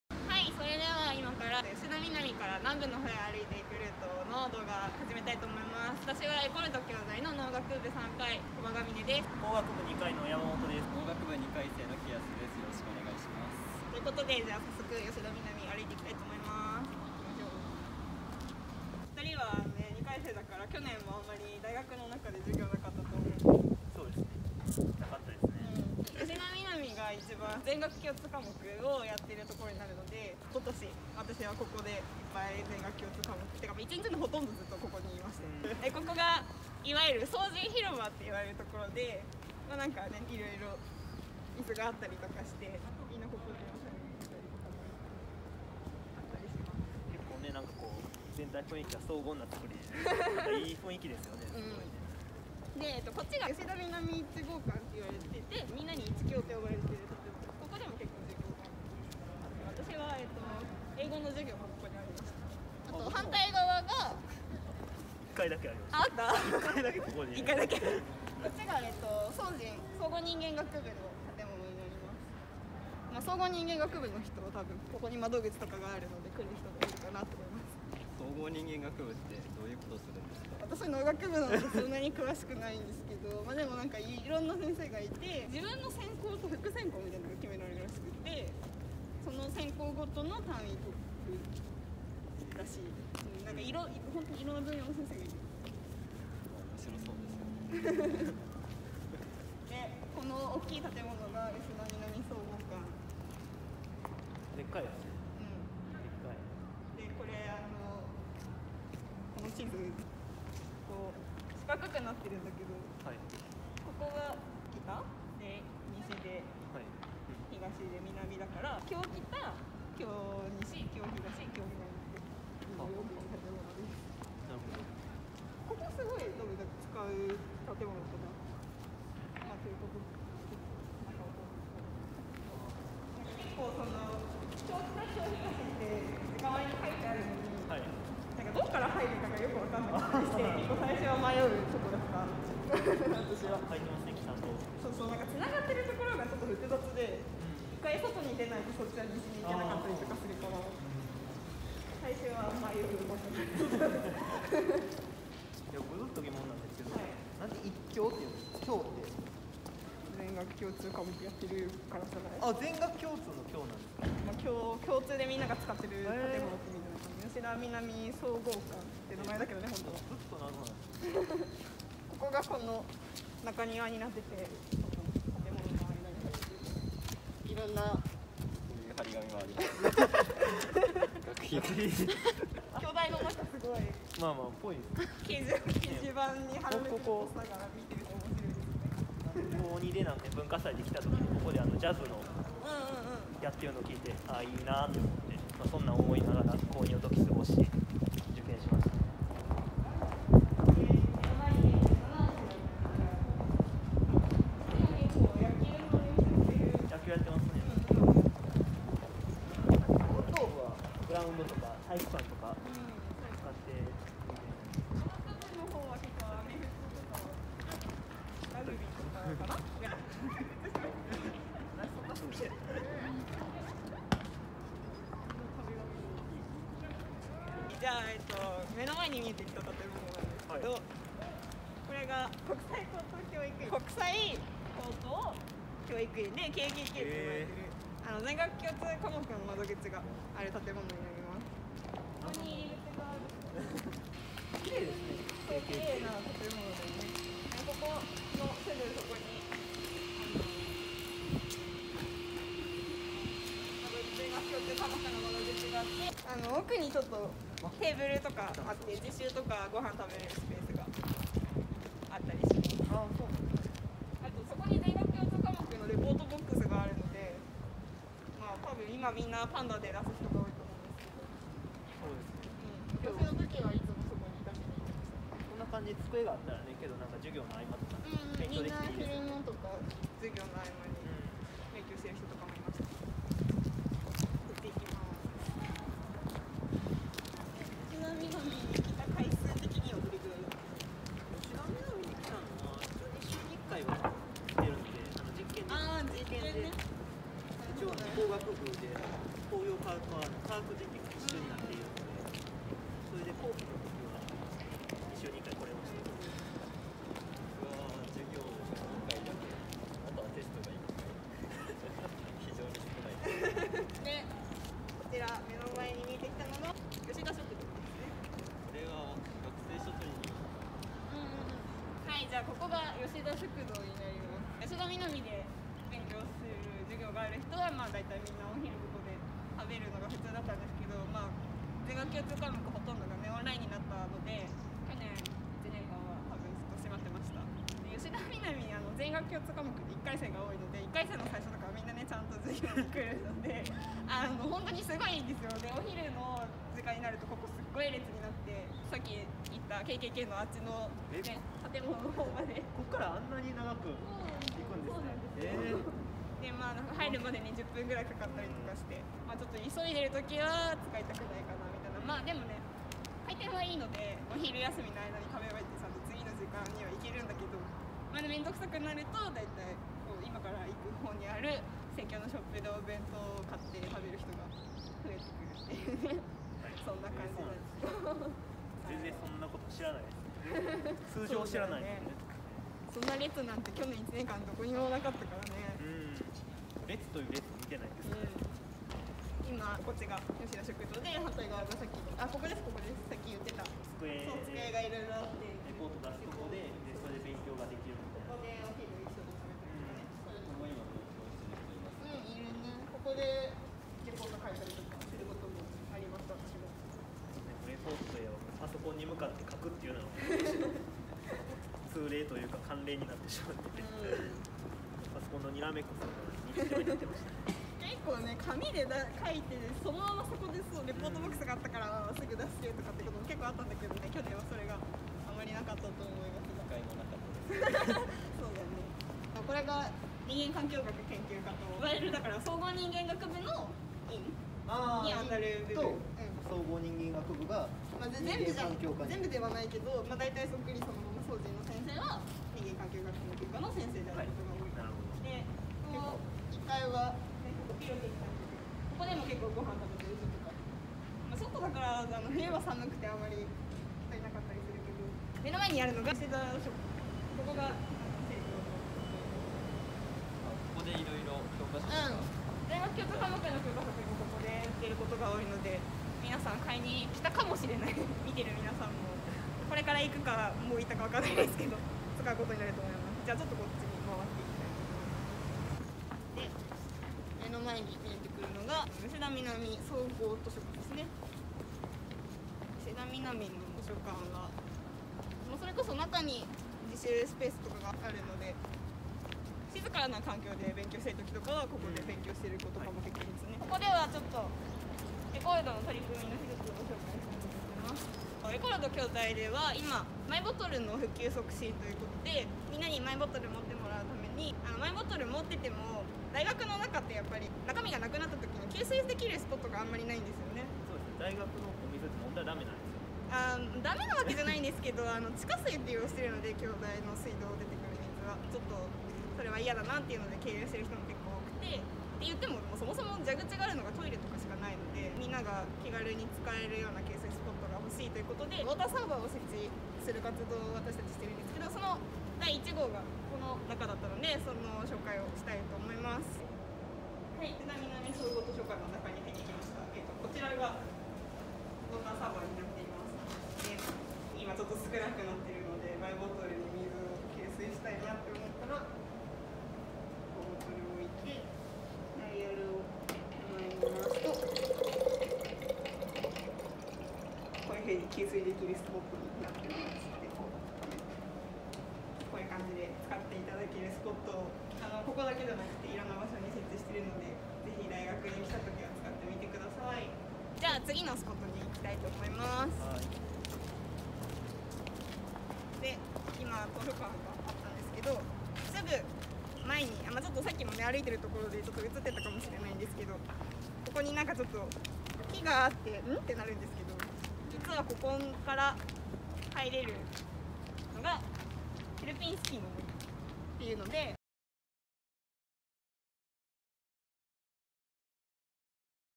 【京都大学SDGsマップ】吉田南〜南部ルートPart.1（吉田南構内正門〜吉田南総合図書館）
毎年エコ〜るど京大が京都大学の新入生向けに作成している環境早見表。
2021年度は「京都大学SDGsマップ」！
京大内のSDGsに関する活動や事例を紹介するマップです。
先生、イベント、建築物…などなど内容は盛りだくさん！

この動画では、エコ〜るど京大メンバーが実際にキャンパス内を歩きながら、
マップの掲載事例はもちろんのこと、よく使う建物などを紹介していきます！
京大生ならではのトークも必見⁉︎

SDGsの達成は一人ではできません。この「京都大学SDGsマップ」によって、
SDGsに興味を持っている人々が活動できる場や人とのつながりをつくることができますように。

今回は吉田南構内を正門からスタートし、吉田南総合図書館までを紹介します！
全学共通科目の授業でほとんどの京大生が使う構内を一緒に見てみましょう〜

0:00　オープニング＠吉田南構内正門
0:35　総人広場
0:53　吉田南1号館（1共）
1:08　総合人間学部棟
1:40　吉田南総合館（共北、共西、共南、共東）
3:20　吉田南グラウンド
3:48　国際高等教育院
5:15　吉田食堂
7:09　人間・環境学研究科棟
7:35　吉田ショップ
7:57　吉田南総合図書館
8:18　ウォーターサーバー